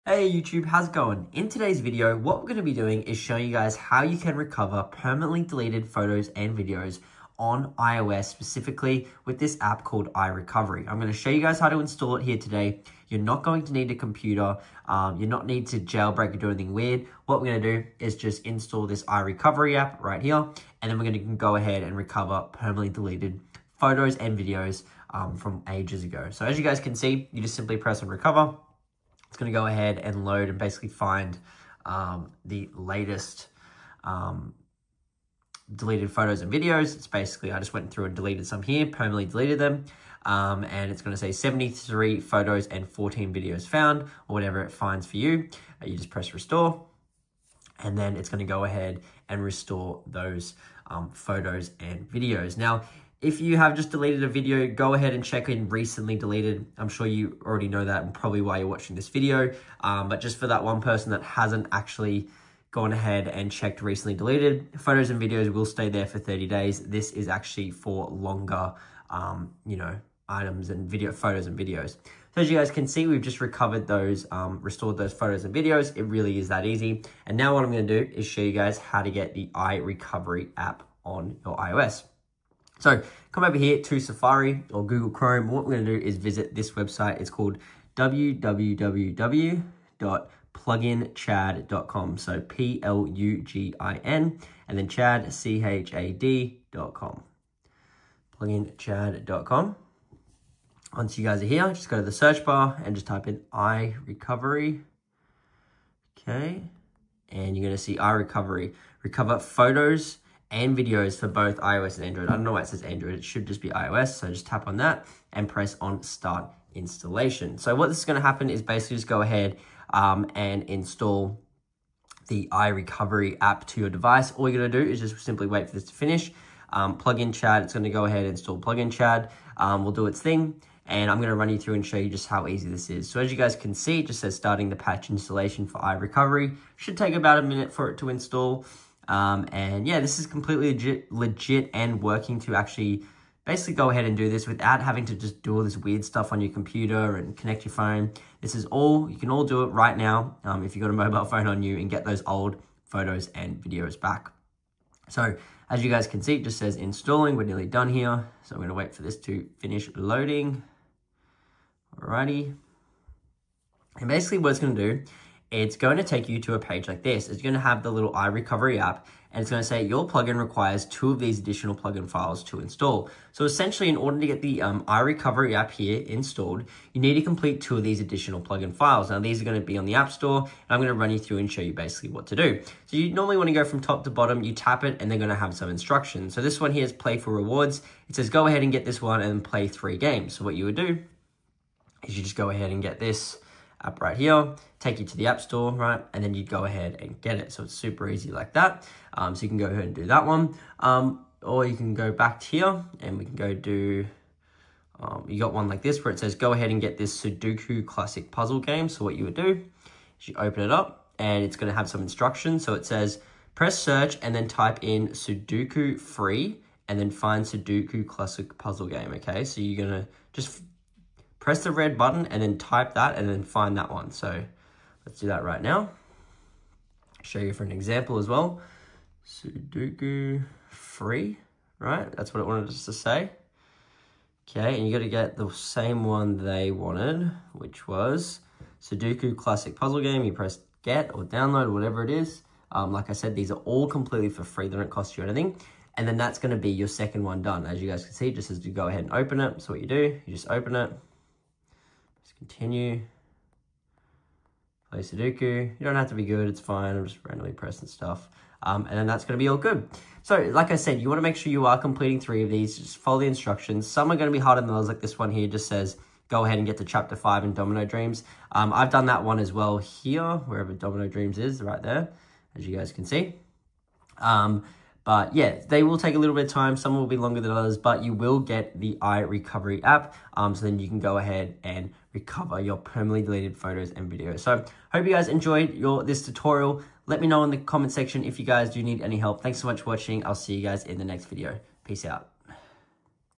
How to Recover Permanently Deleted Photos & Videos on iOS iPhone iPad 2025! (NO COMPUTER)
How to Recover Permanently Deleted Photos & Videos on iOS iPhone/iPad 2025!

Lost your precious photos or videos on your iPhone or iPad? Don’t worry! In this video, we’ll guide you through the latest methods to recover permanently deleted photos and videos on iOS in 2025. Whether they were accidentally deleted or lost due to a device reset, we’ve got you covered with step-by-step instructions.

📌 In this video, you’ll learn:

How to check for recently deleted items in the Photos app
Using iRecovery to get them back
Advanced recovery tools and software for iOS